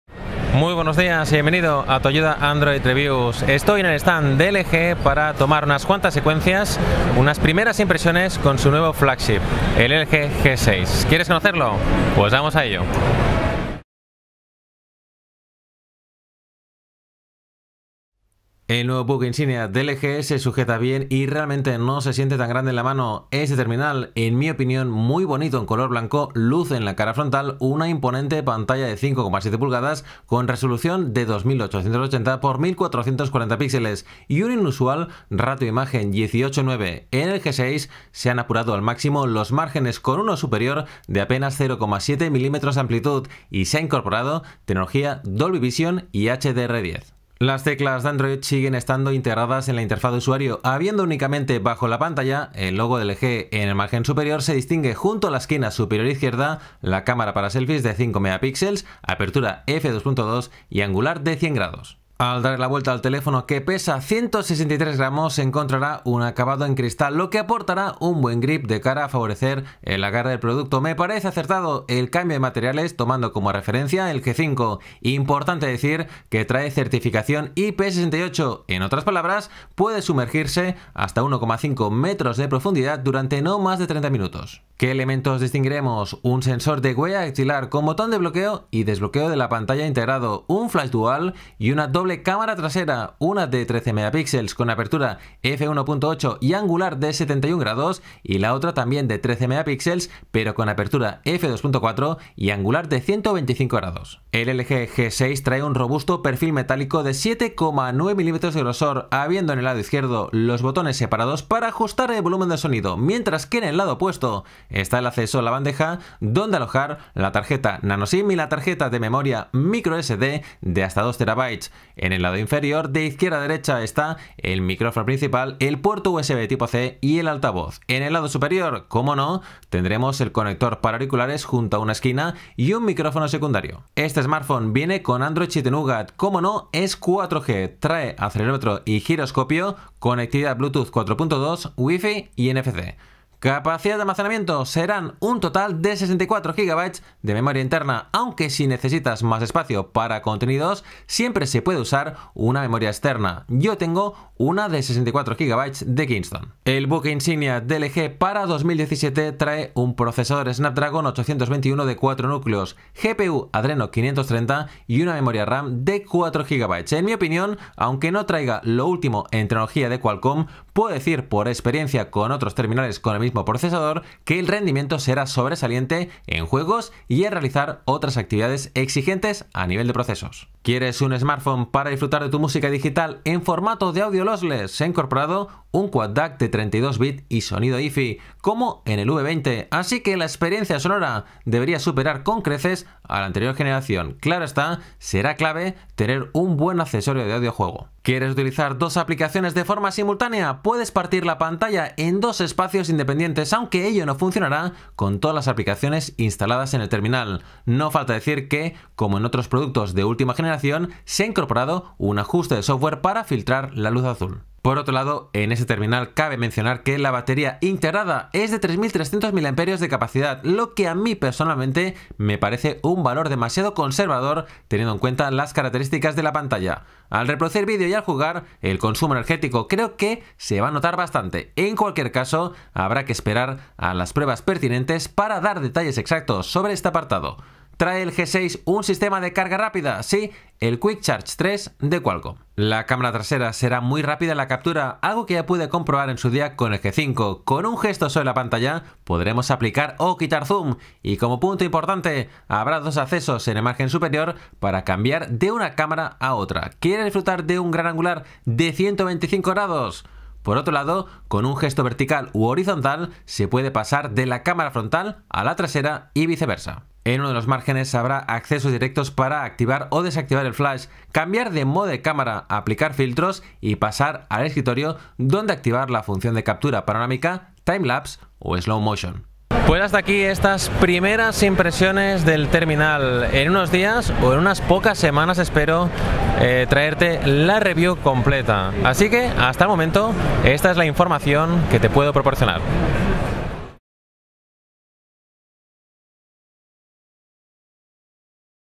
LG G6 | Primeras impresiones en español (MWC 17)
LG G6, primeras impresiones en español. Review de características del nuevo flagship de la marca que, como no, traerá Android 7 Nougat ya pre-instalado.

El LG G6 contará con dos apartados clave: la doble cámara trasera de 13MP, pero con angular distinto de 71º y 125º; y la inusual pantalla de 5,7" y resolución de 2880x1440 pixeles.

¿Cuál es una de las características que puede darle debilidad al terminal? La batería de 3300 mAh de capacidad: en mi opinión demasiado justa teniendo en cuenta las características de la pantalla.

En unas pocas semanas tendrás disponible la review completa de este teléfono móvil anunciado durante el Mobile World Congress 2017 de Barcelona.

Sigue a Tu Ayuda Android Reviews para estar al día de los últimos tutoriales, unboxings y reviews exclusivas de productos de tecnología con sistema operativo Android: